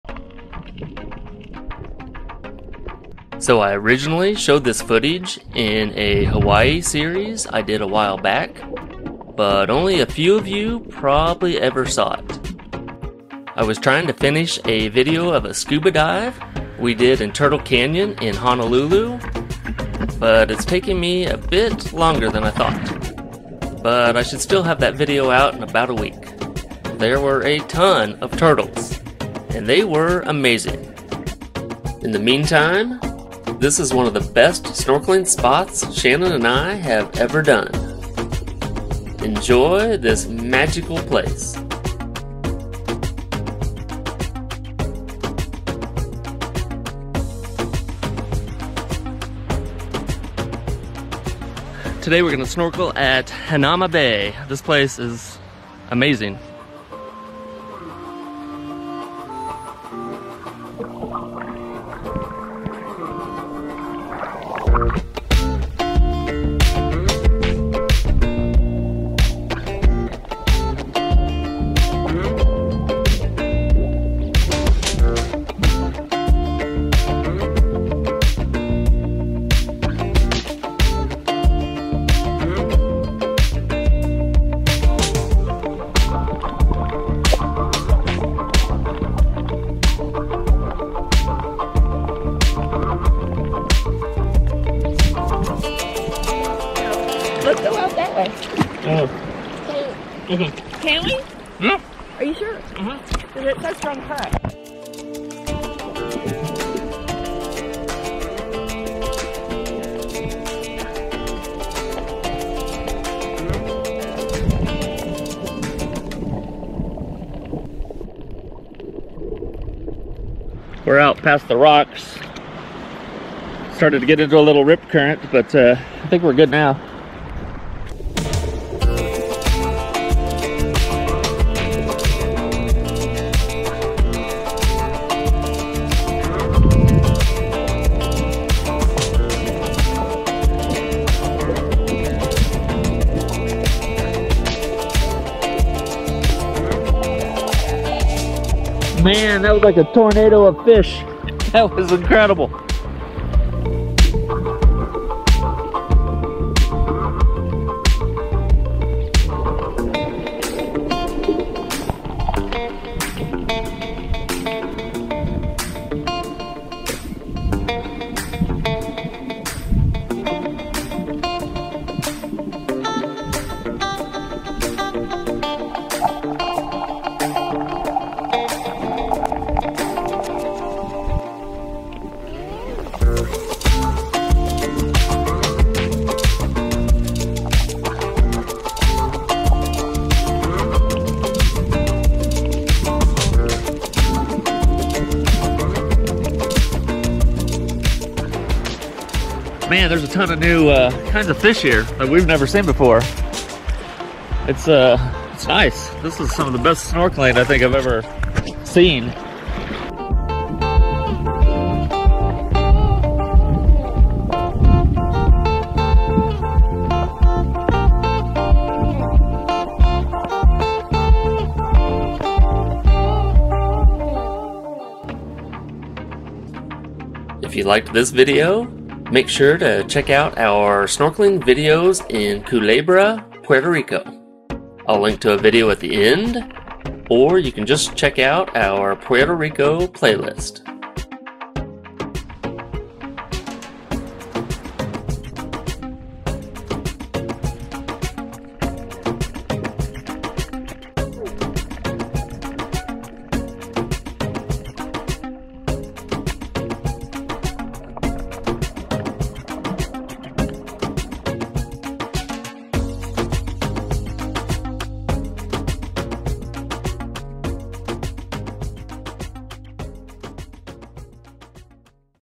Unbelievable Snorkeling at Hanauma Bay Oahu, Hawaii
The snorkeling was amazing at Hanauma Bay in Oahu, Hawaii! We even saw what looked like a tornado of fish! Snorkeling there is a must do if you are in the area. So many pretty fish!